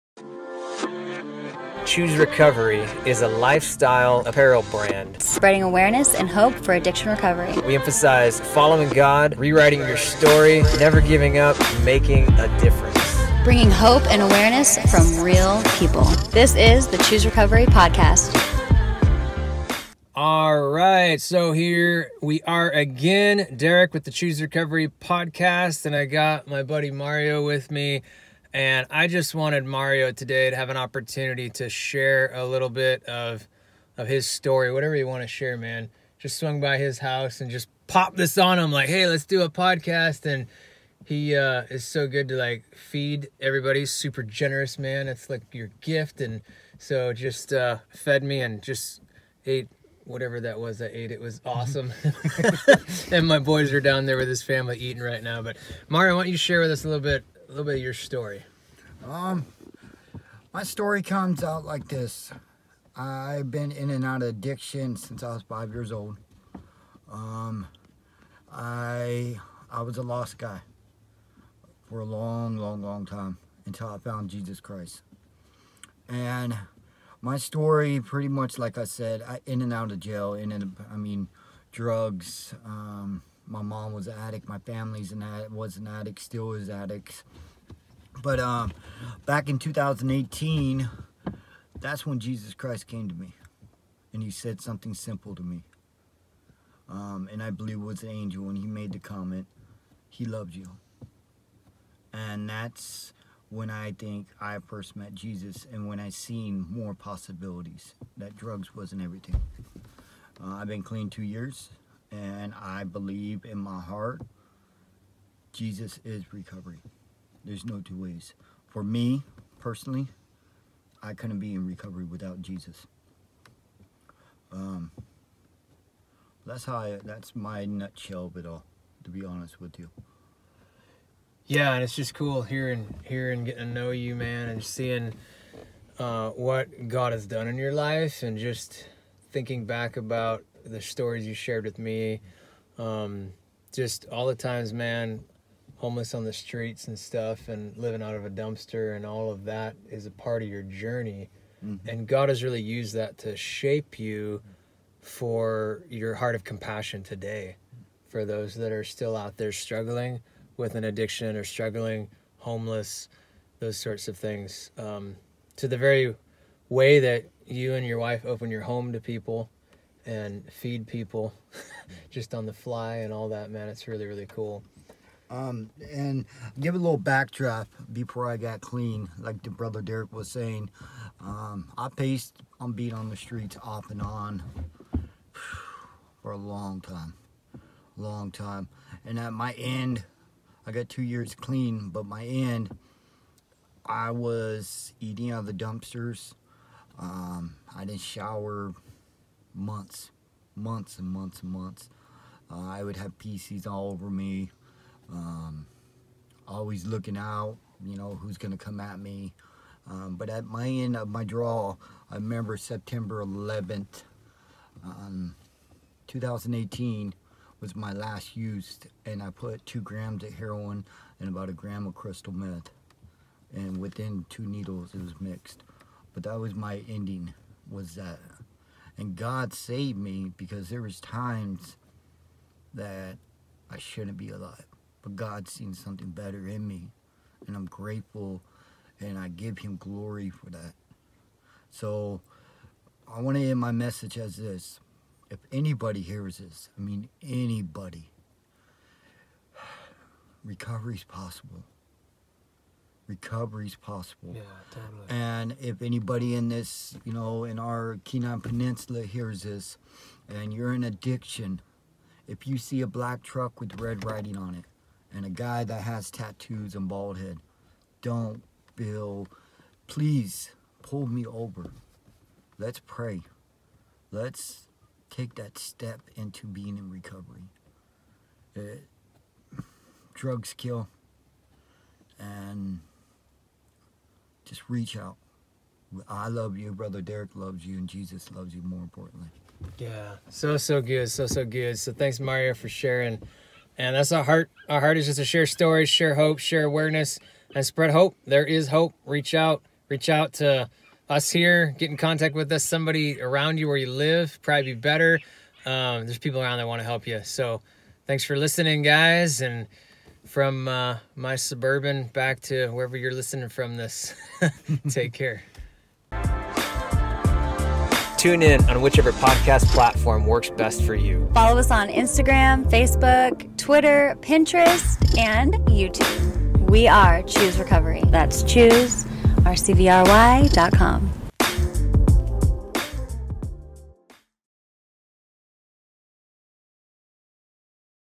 Real Life Stories: Mario Choose RCVRY Podcast
My buddy Mario joins the Choose RCVRY Podcast to share his story of recovery!